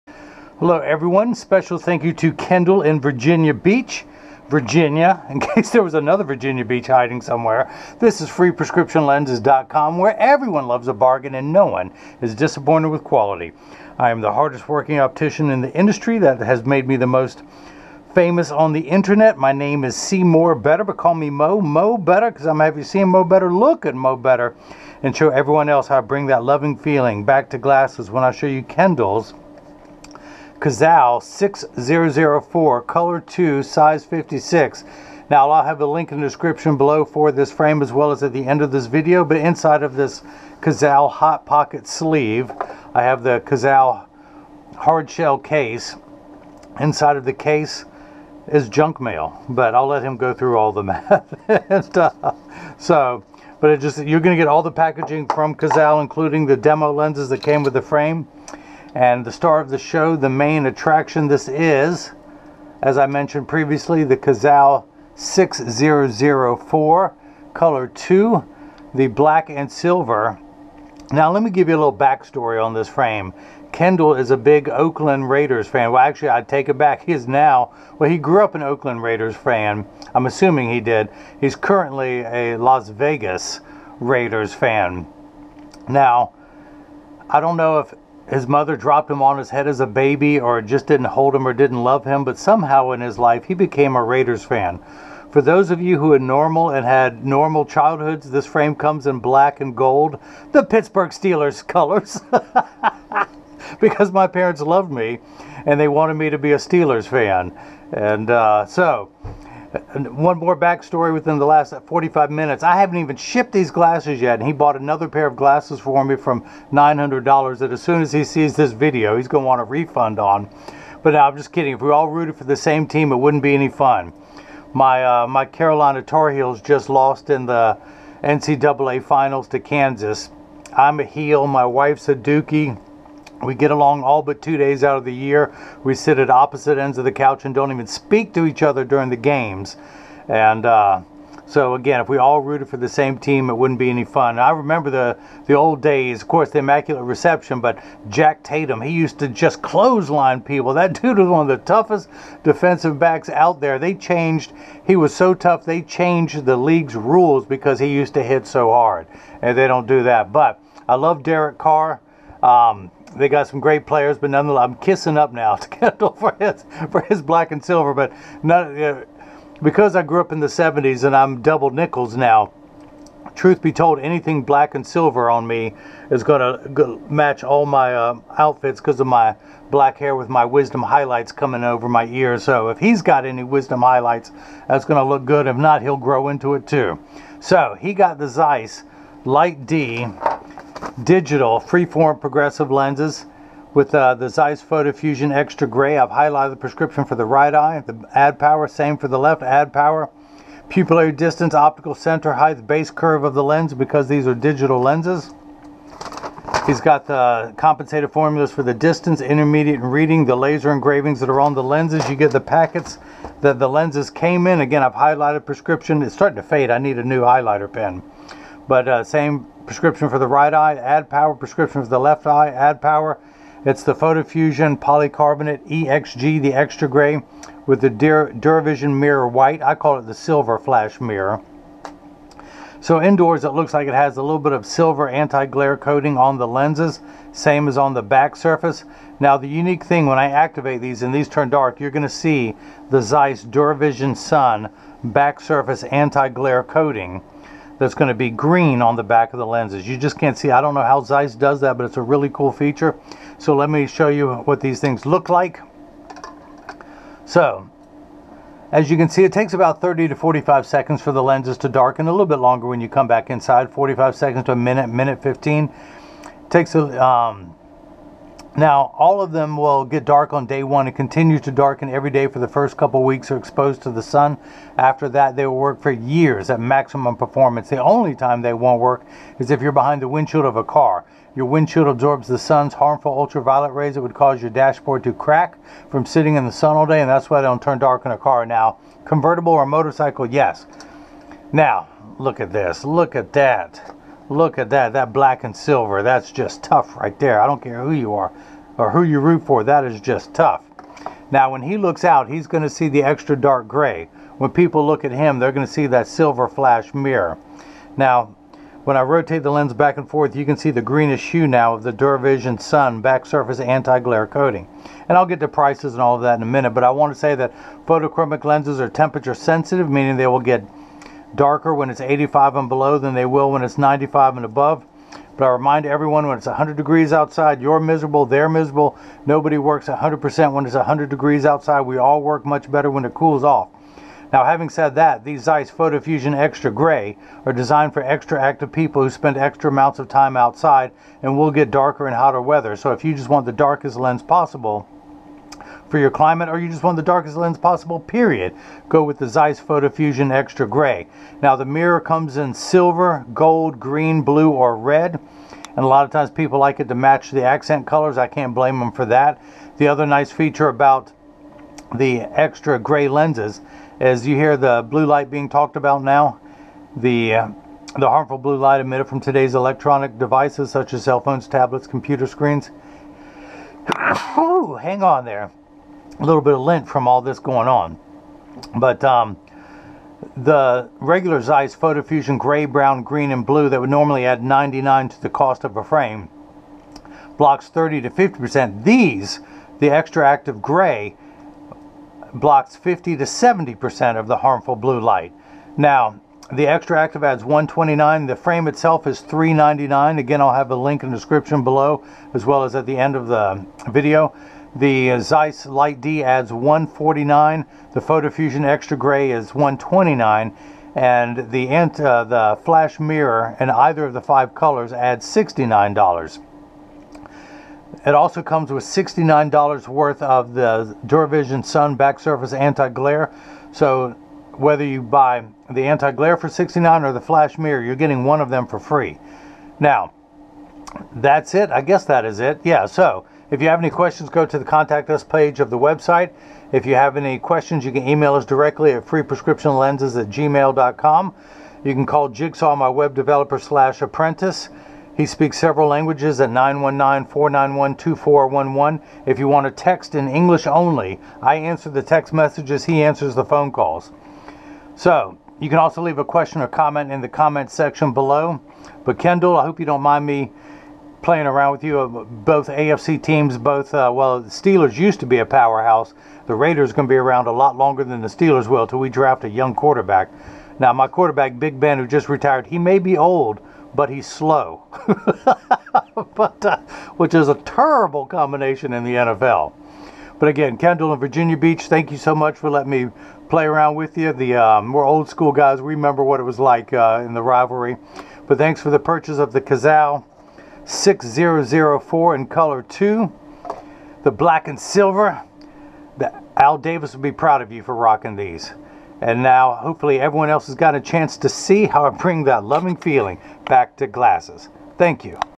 Kendall's Cazal 6004 | Zeiss Photofusion Extra Grey With Silver Flash Mirrors
Link to this frame:
Watch as a show you the Cazal 6004 Black and Silver frames with Zeiss Photofusion Extra Active Grey with Silver Flash Mirrored Lenses. Kendall in Virginia Beach, VA is about to see mo better.